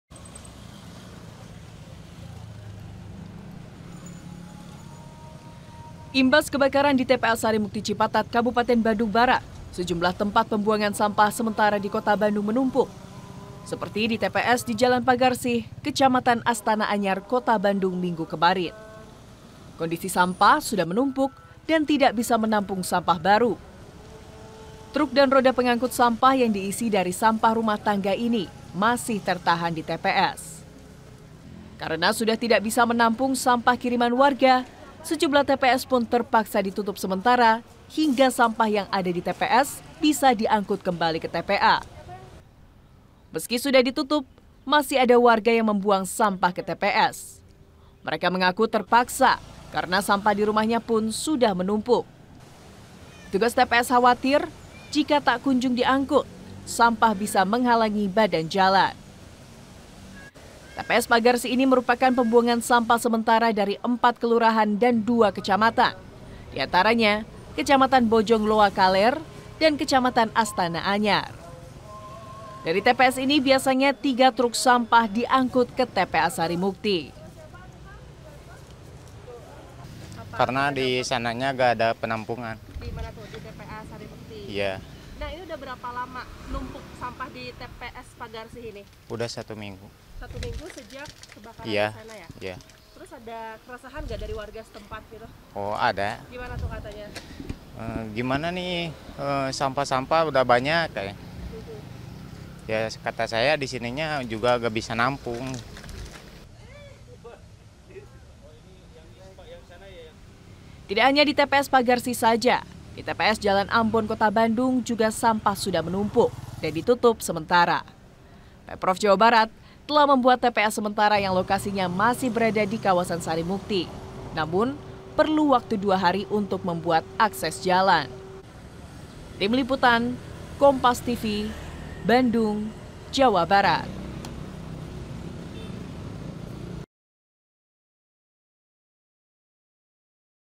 Imbas Kebakaran TPS Sarimukti, Sampah di Bandung Menumpuk
BANDUNG, KOMPAS.TV - Imbas kebakaran di TPA Sarimukti Cipatat Kabupaten Bandung Barat, sejumlah Tempat Pembuangan Sampah sementara di Kota Bandung menumpuk. Seperti di TPS di Jalan Pagarsih Kecamatan Astana Anyar Kota Bandung minggu kemarin. Kondisi sampah sudah menumpuk dan tidak bisa menampung sampah baru.

Truk dan roda pengangkut sampah yang diisi dari sampah rumah tangga ini masih tertahan di TPS. Karena sudah tidak bisa menampung sampah kiriman warga, sejumlah TPS pun terpaksa ditutup sementara hingga sampah yang ada di TPS bisa diangkut kembali ke TPA. Meski sudah ditutup, masih ada warga yang membuang sampah ke TPS. Mereka mengaku terpaksa, karena sampah di rumahnya pun sudah menumpuk. Petugas TPS khawatir jika tak kunjung diangkut, sampah bisa menghalangi badan jalan.

TPS Pagarsih ini merupakan pembuangan sampah sementara dari empat kelurahan dan dua kecamatan, di antaranya Kecamatan Bojong Loa Kaler dan Kecamatan Astana Anyar, dari TPS ini biasanya tiga truk sampah diangkut ke TPA Sarimukti.

Tidak hanya di TPS Pagarsih saja, di TP Jalan Ambon Kota Bandung juga sampah sudah menumpuk dan ditutup sementara. Pemprov Jawa Barat telah membut TPA sementara yang lokasinya masih berada di kawasan Sarimukti, namun perlu waktu dua hari untuk membuat akses jalan.

Untuk lebih tahu berita terupdate seputar Jawa Barat, bisa klink link di bawah: